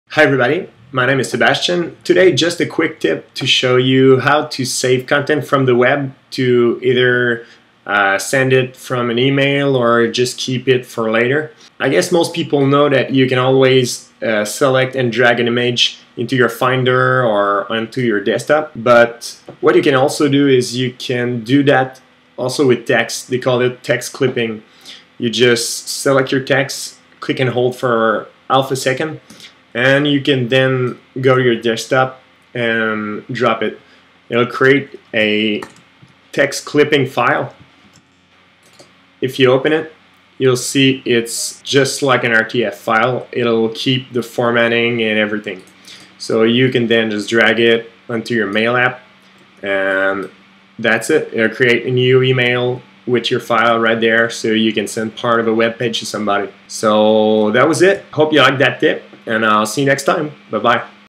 How to Create a Quick Text Clipping
A quick tip explaining how to create a new file from a text selection without having to copy and paste into a new file.